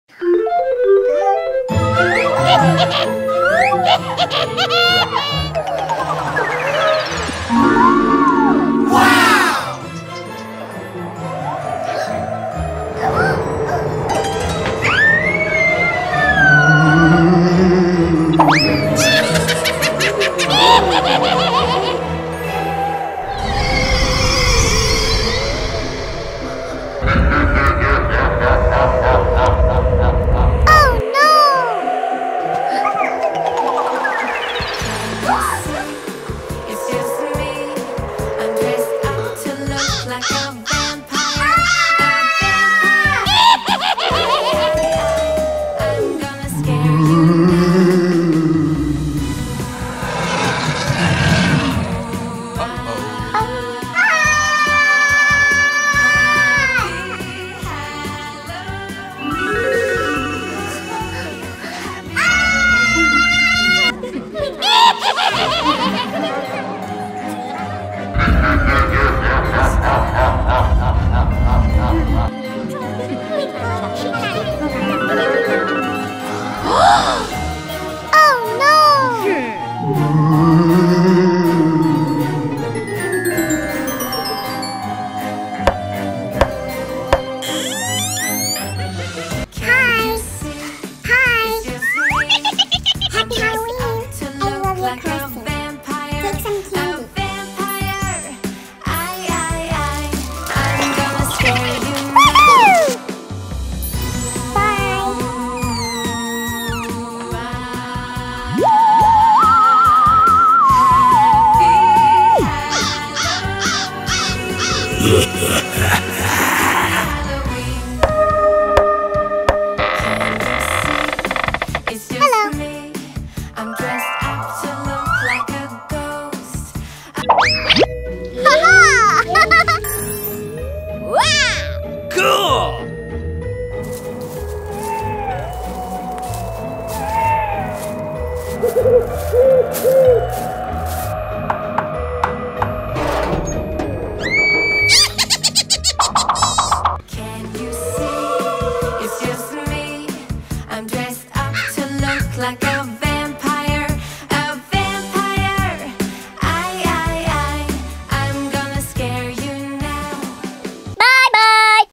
Niki and Makko and mysterious adventures on Halloween
Nika and Diana and her mysterious Halloween-themed stories for kids.